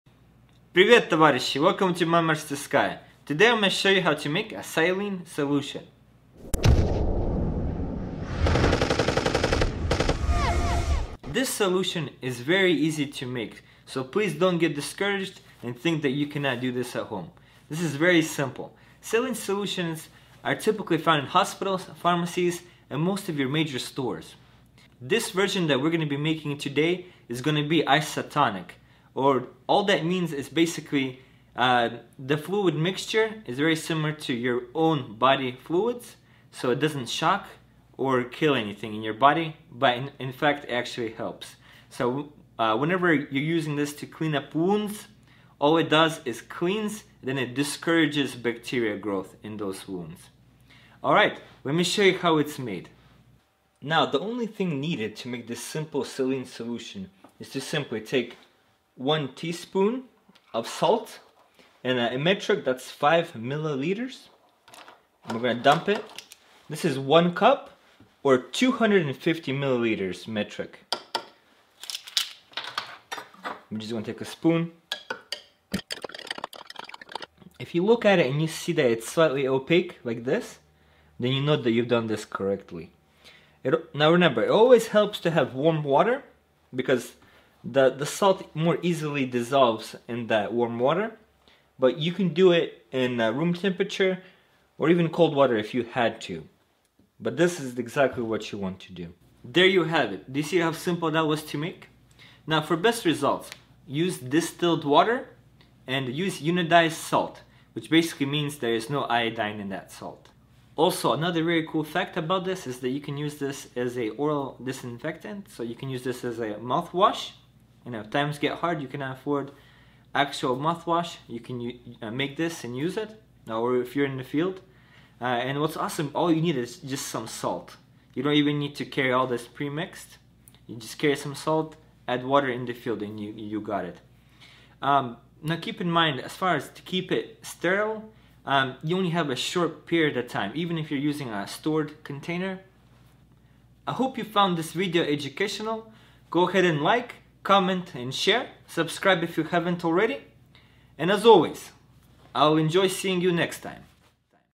DIY Saline Solution
Saline solution refers to a salt solution, which you can prepare yourself using readily available materials. The solution can be used as a disinfectant or sterile rinse. This recipe is for a salt solu